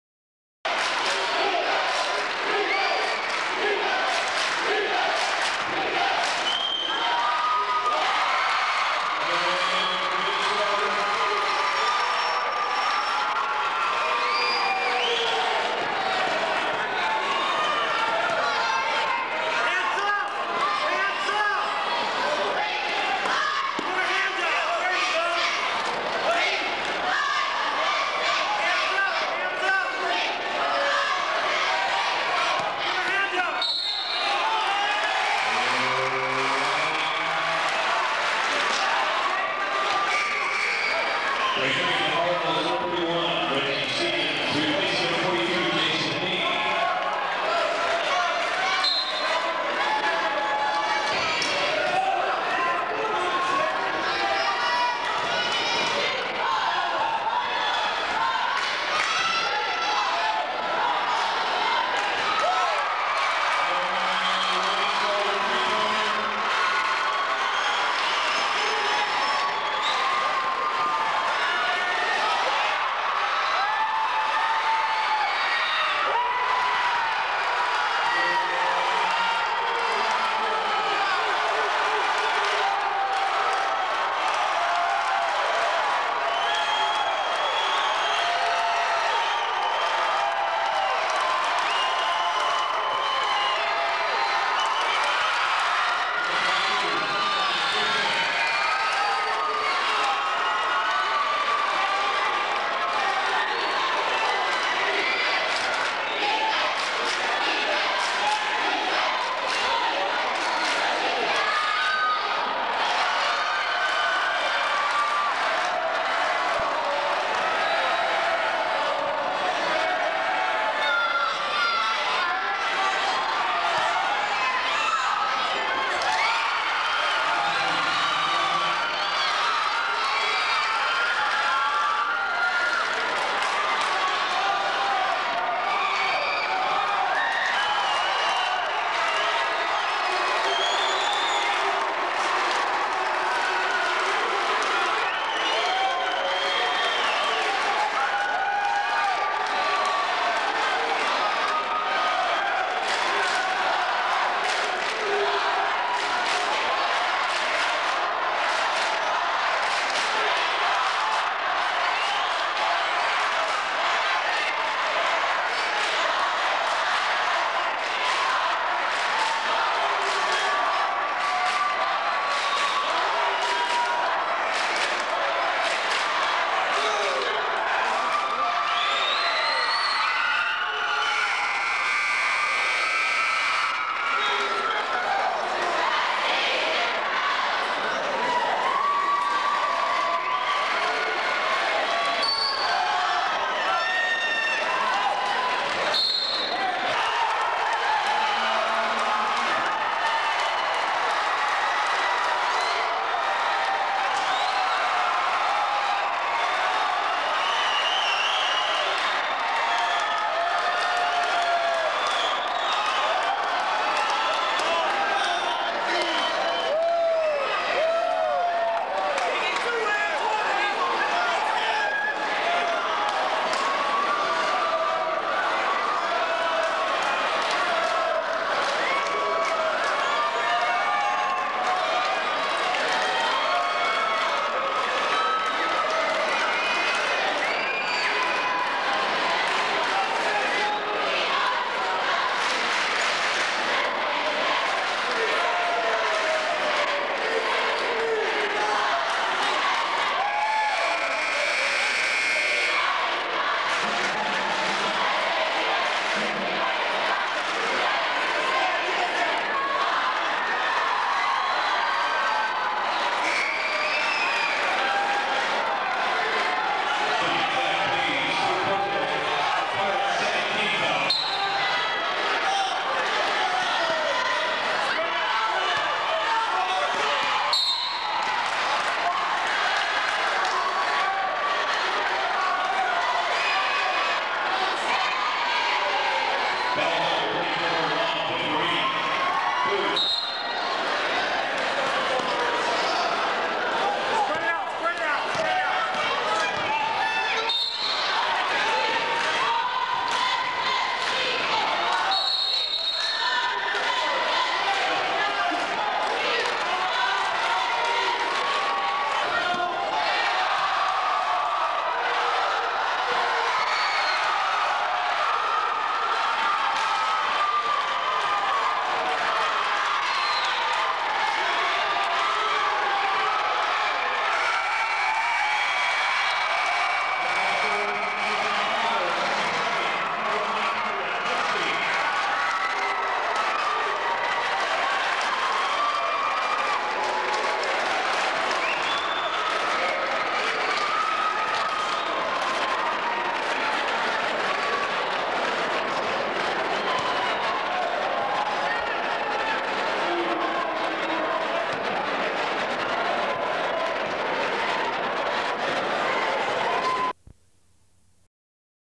January 21,1992 Santa Cruz High v Harbor Basketball DHM #16
Vanessa Dunn singlehandedly cheers team to victory